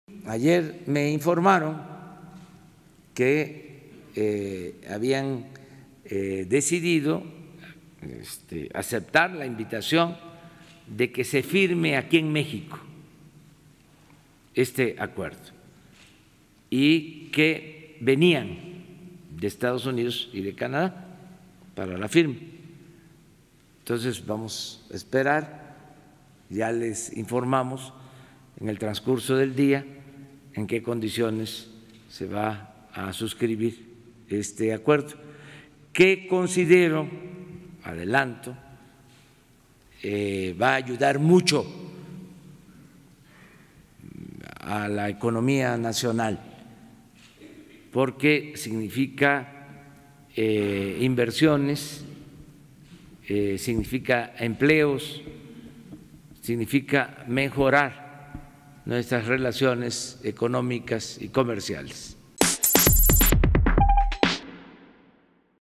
Acepta Canadá y EU invitación para firmar en México el T-MEC: AMLO
En conferencia mañanera, el presidente Andrés Manuel López Obrador sostuvo que el tratado va a ayudar mucho a la economía nacional, "significa inversiones, empleos, significa mejorar nuestras relaciones económicas y comerciales."